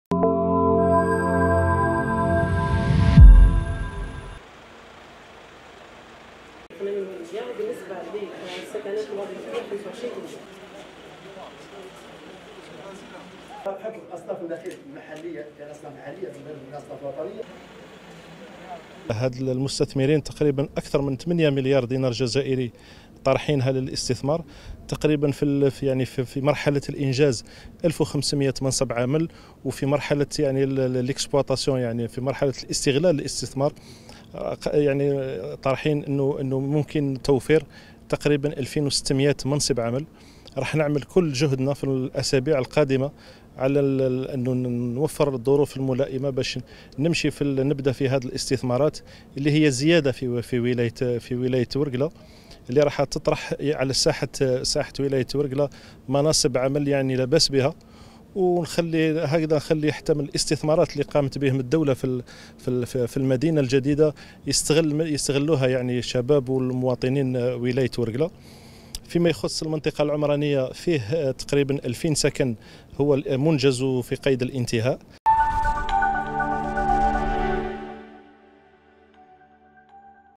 بلعريبي في زيارة عمل و تفقد لمشاريع تابعة للقطاع بالمدينة الجديدة لحاسي مسعود
ورقلة (وأج)- أكد وزيرالسكن طارق محمد بلعريبي خلال زيارته التفقدية للمدينة الجديدة بحاسي مسعود أن ورشات برنامج 2000 سكن الجاري إنجازها ضمن المخطط العمراني للمدينة الجديدة يشهد تقدما ملحوظا.